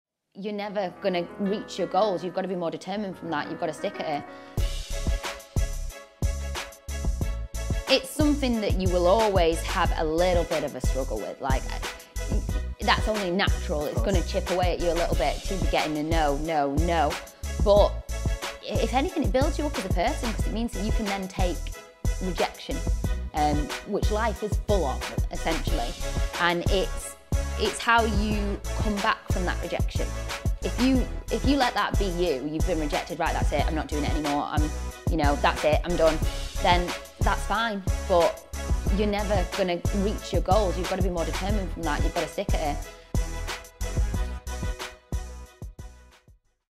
Mini Insights | Actress MOLLIE WINNARD | Actor Motivation | "Sticking With It"
Ignite Films Presents - Mini Insights

Mollie is an award winning actress from her role on Coronation Street and has now taken a role in the acclaimed Channel 5 Drama All Creatures Great and Small.

Mini Clips that proved direct answers to some of the industrys most asked questions. We will bring you advice and answers from Directors, Actors, Casting Directors, Agents, Photographers and more.